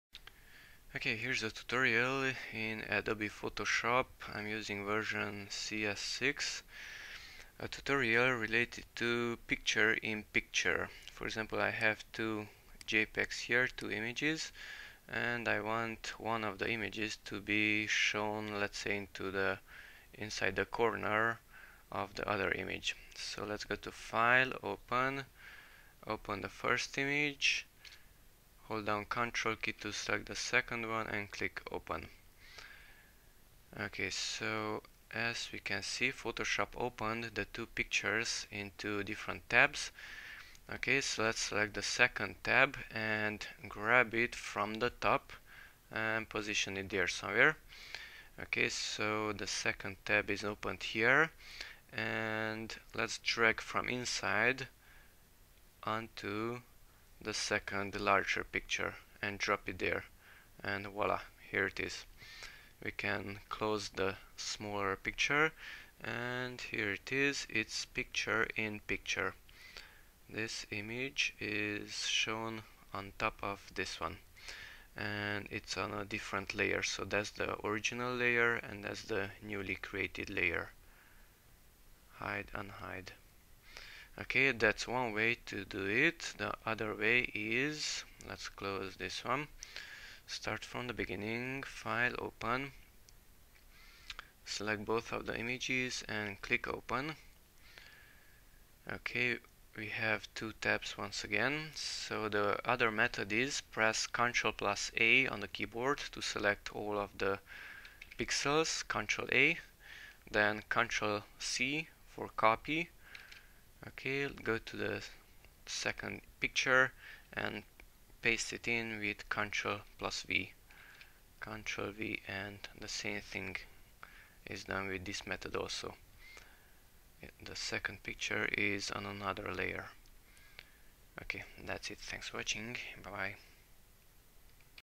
How to do Picture In Picture in Photoshop
How to put one picture into another picture using Adobe Photoshop